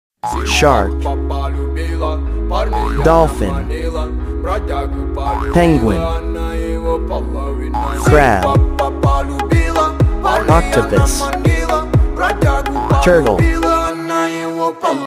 CUTE AND ADORABLE ANIMALS 🥰 SEA ANIMALS
A video of cute and adorable sea animals. can make people who see it like it.